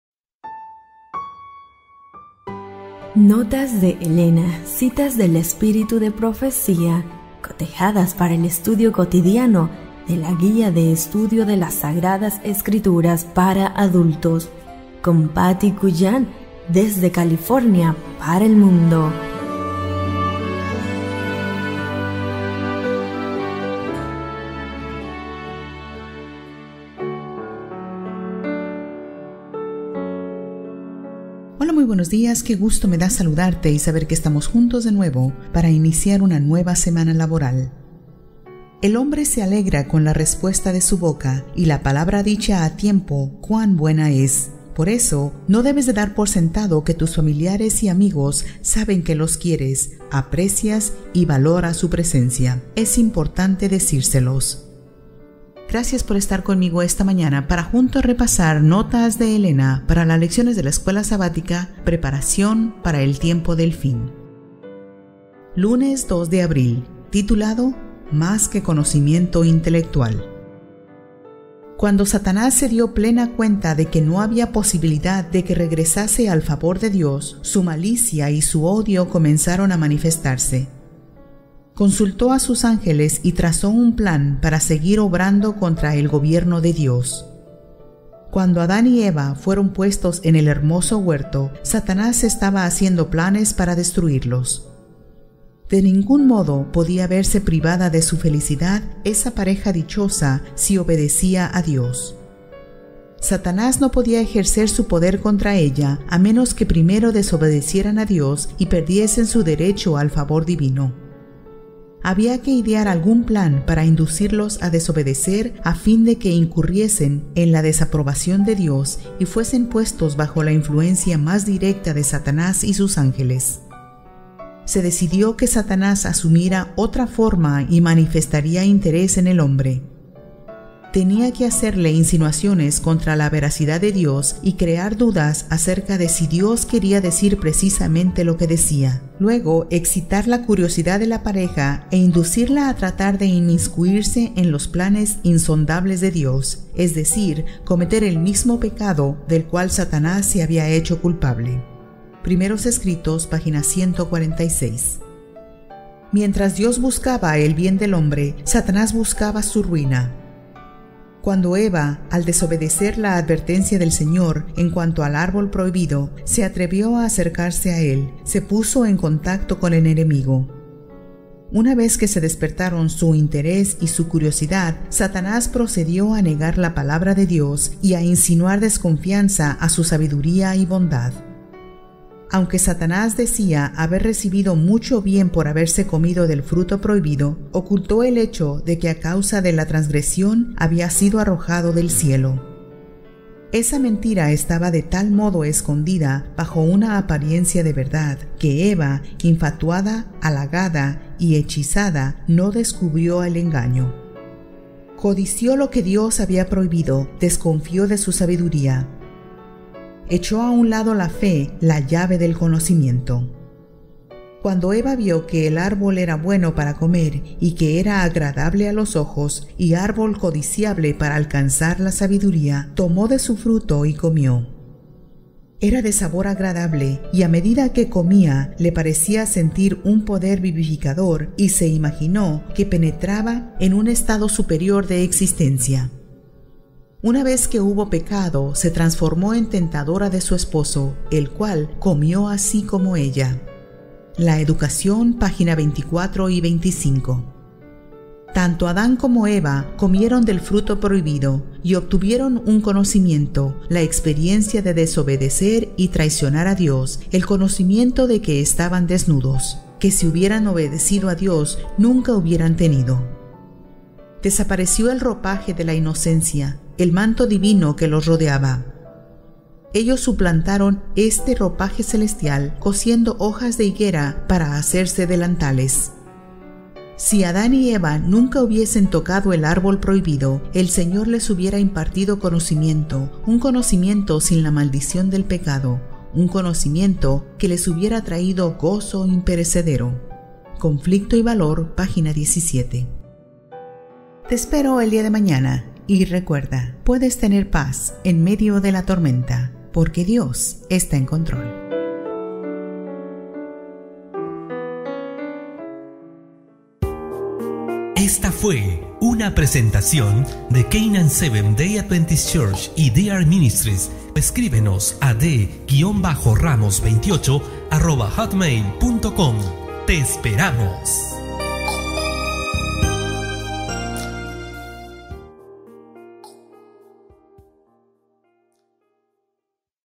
Notas de Elena - 02 de Abril del 2018 - Más que conocimiento intelectual
MÁS QUE CONOCIMIENTO INTELECTUAL
Cuando Satanás se dio plena cuenta de que no había posibilidad de que regresase al favor de Dios, su malicia y su odio comenzaron a manifestarse. Consultó a sus ángeles, y trazó un plan para seguir obrando contra el gobierno de Dios. Cuando Adán y Eva fueron puestos en el hermoso huerto, Satanás estaba haciendo planes para destruirlos. De ningún modo podía verse privada de su felicidad esa pareja dichosa si obedecía a Dios. Satanás no podía ejercer su poder contra ella a menos que primero desobedeciesen a Dios y perdiesen su derecho al favor divino. Había que idear algún plan para inducirlos a desobedecer a fin de que incurriesen en la desaprobación de Dios y fuesen puestos bajo la influencia más directa de Satanás y sus ángeles. Se decidió que Satanás asumiría otra forma y manifestaría interés en el hombre. Tenía que hacerle insinuaciones contra la veracidad de Dios y crear dudas acerca de si Dios quería decir precisamente lo que decía; luego, excitar la curiosidad de la pareja e inducirla a tratar de inmiscuirse en los planes insondables de Dios -es decir, cometer el mismo pecado del cual Satanás se había hecho culpable (Primeros escritos, p. 146).
Mientras Dios buscaba el bien del hombre, Satanás buscaba su ruina. Cuando Eva, al desobedecer la advertencia del Señor en cuanto al árbol prohibido, se atrevió a acercarse a él, se puso en contacto con el enemigo. Una vez que se despertaron su interés y su curiosidad, Satanás procedió a negar la palabra de Dios, y a insinuar desconfianza en su sabiduría y bondad ...
Aunque Satanás decía haber recibido mucho bien por haber comido del fruto prohibido, ocultó el hecho de que a causa de la transgresión había sido arrojado del cielo. Esa mentira estaba de tal modo escondida bajo una apariencia de verdad, que Eva, infatuada, halagada y hechizada, no descubrió el engaño. Codició lo que Dios había prohibido; desconfió de su sabiduría. Echó a un lado la fe, la llave del conocimiento.
Cuando Eva vio que "el árbol era bueno para comer, y que era agradable a los ojos, y árbol codiciable para alcanzar la sabiduría ... tomó de su fruto, y comió". Era de sabor agradable, y a medida que comía, le parecía sentir un poder vivificador y se imaginó que penetraba en un estado superior de existencia. Una vez que hubo pecado, se transformó en tentadora de su esposo "el cual comió así como ella" Génesis 3:6 (La educación, pp. 24, 25).
Tanto Adán como Eva comieron del fruto prohibido, y obtuvieron un conocimiento -la experiencia de desobedecer y traicionar a Dios, el conocimiento de que estaban desnudos- que, si hubieran obedecido a Dios, nunca hubieran tenido. Desapareció el ropaje de la inocencia, el manto divino que los rodeaba. Ellos suplantaron este ropaje celestial cosiendo hojas de higuera para hacerse delantales ...
Si Adán y Eva nunca hubiesen tocado el árbol prohibido, el Señor les hubiera impartido conocimiento: un conocimiento sin la maldición del pecado, un conocimiento que les hubiera traído gozo imperecedero (Conflicto y valor, p. 17).